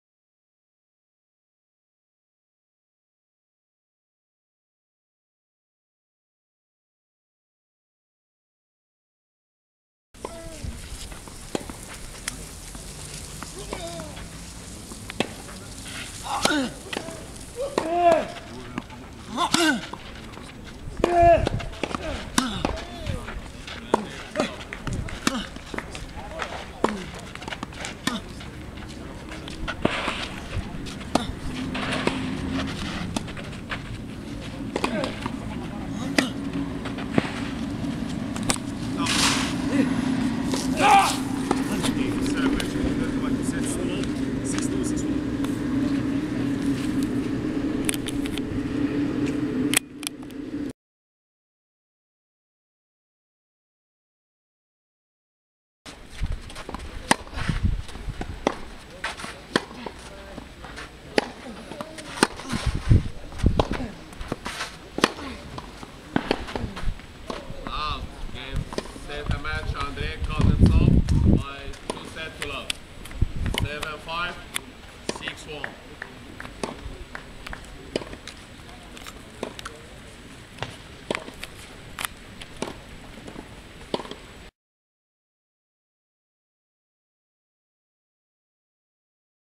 TA - Egypt F1 Futures Semi Finals (Tennis)
This is a video showing the match point of the two semi final matches in Egypt F2 Futures. This first match is between Recouderc and Kirche and Recouderc won 62 61. The second match is between Kuzntesov and Safwat and Kuznetsov won 75 61.